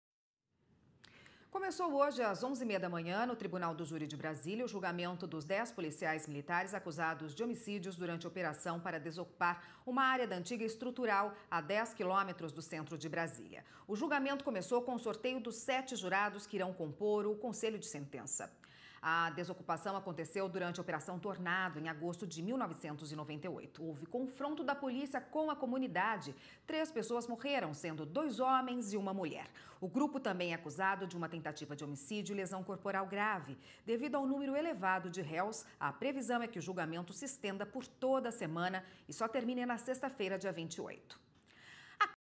Caso Estrutural (24/08/15)
Começou hoje as onze e meia da manhã, no tribunal do júri de Brasília o julgamento dos  policiais militares acusados de homicídios durante operação para desocupar área da antiga estrutural, a dez quilômetros do centro de Brasília. O julgamento começou com o sorteio dos sete jurados que irão compor o conselho de sentença. A desocupação, aconteceu durante a operação Tornado, em agosto de 1998. Houve confronto da polícia com a comunidade. Três pessoas morreram, sendo dois homens e uma mulher. O grupo  também é acusado de uma tentativa de homicídio e lesão corporal grave. Devido ao número elevado de réus, a previsão é que o julgamento se estenda por toda a semana e só termine na sexta-feira, dia 28.